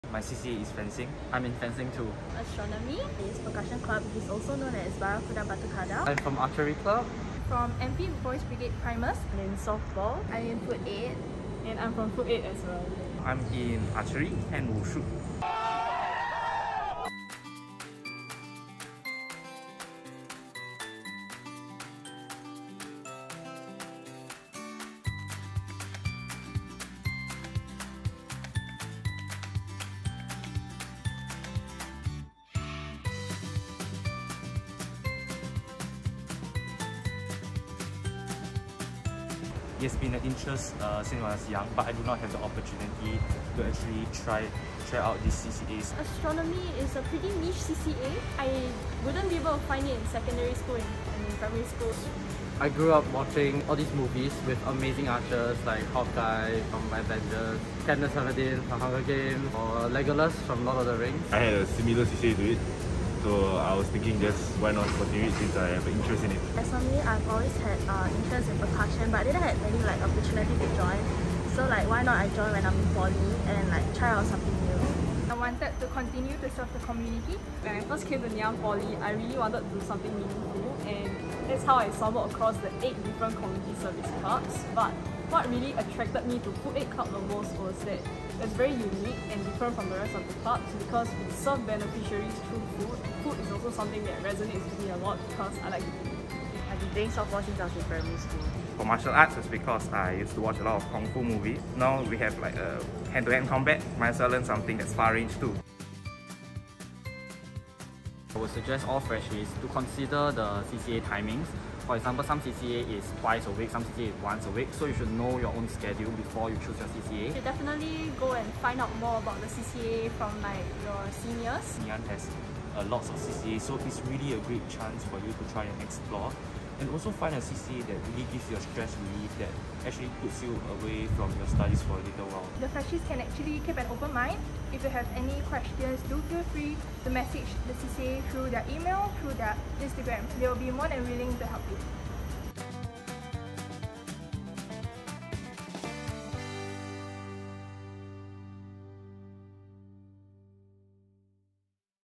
Chatterbox with NP: How to Choose Your CCA
Thinking about which CCA to join? 🤔 Let our seniors share their CCA experiences and useful tips on choosing the right CCA for you! and book the try-outs to get a taste of the activities!